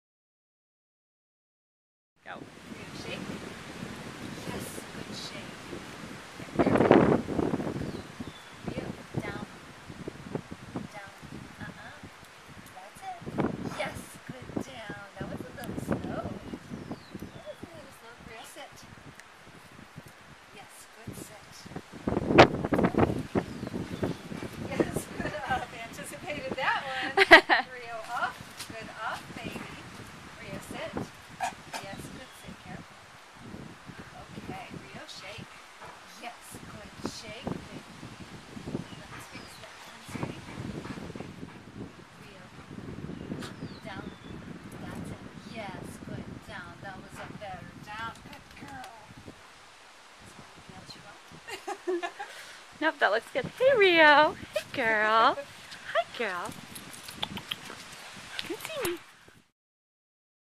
Wisteria Goldens English Cream Golden Retriever Puppies Rio Born 02-02-10 | WisteriaGoldens.com
Faith/Buddy - Rio - Born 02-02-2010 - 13 Weeks Old at Wisteria Ranch English Creme Golden Retriever Puppies For Sale